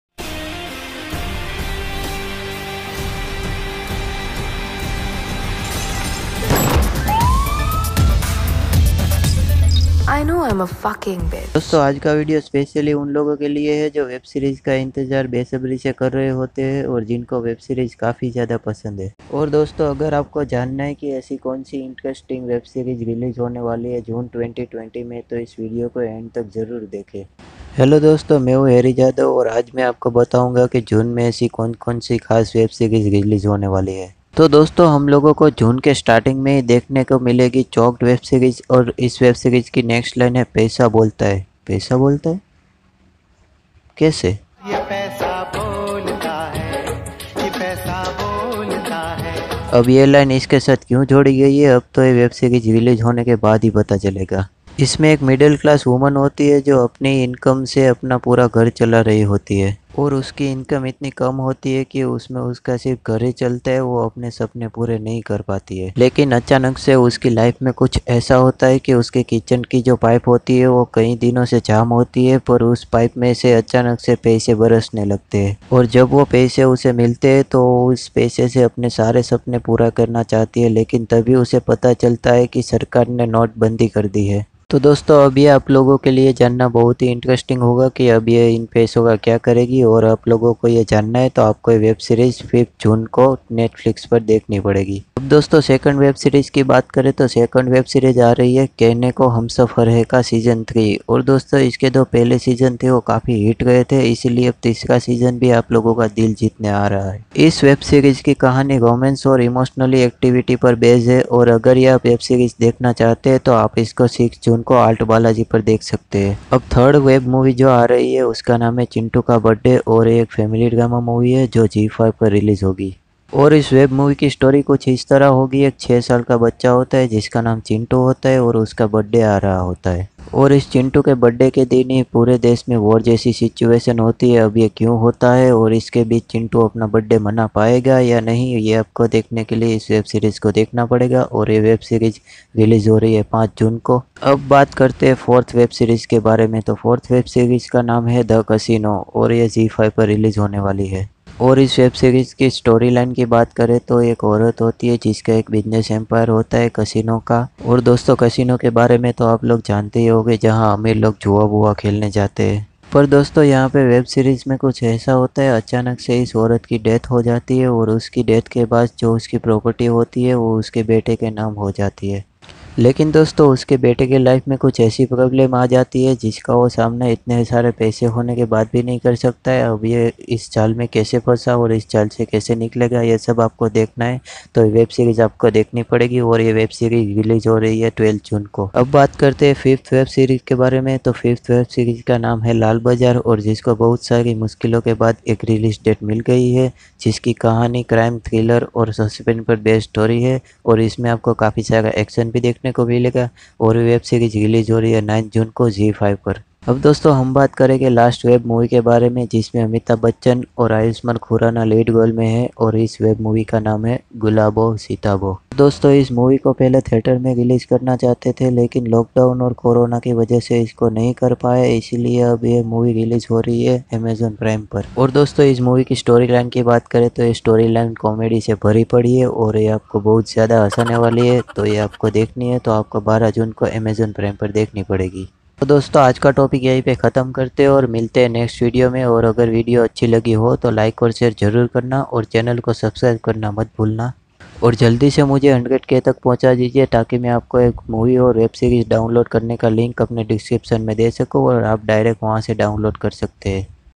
Upcoming Web Series June 2020 | New Web Series
Upcoming Web Series tell by Harry Jadav

Best hindi web series release on June 2020 all web series available in hindi and the best web series list is created by me so please watch the video and don't forget to subscribe my channel for more information about the best web series and movies